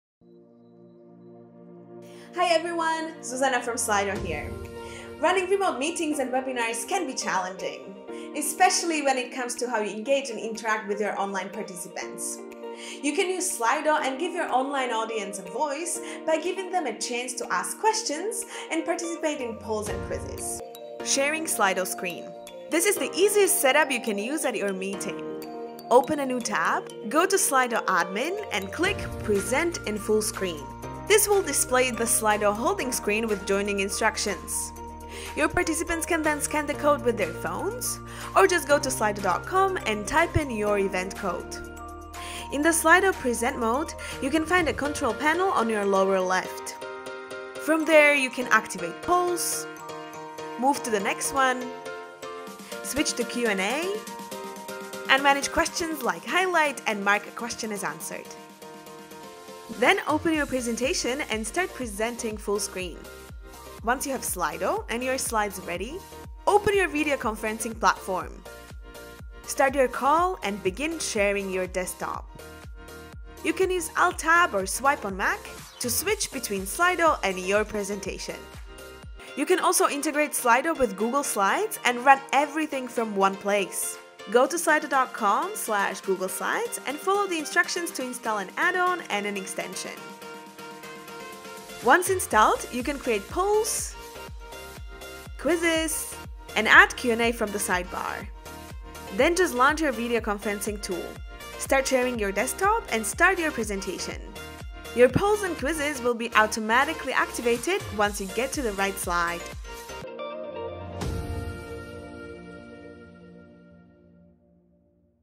How to Use Slido at Your Remote Meeting (Old)
Running remote meetings and webinars can be challenging. Especially when it comes to how you engage and interact with your audience. You can use Slido and give your online audience a voice by giving them a chance to ask questions or participate in polls. Here is a simple setup of Slido for your remote meeting. There are three options:

1/ Sharing the Slido screen  - The easiest setup you can use at your meeting.
2/ Slido Switcher - A good option if you have another person helping you with Slido or you want to use your smartphone as a remote control.
3/ Integration with Google Slides - Run everything from one place.

And that’s it.

Good luck with engaging your remote participants at your next meeting or  webinar.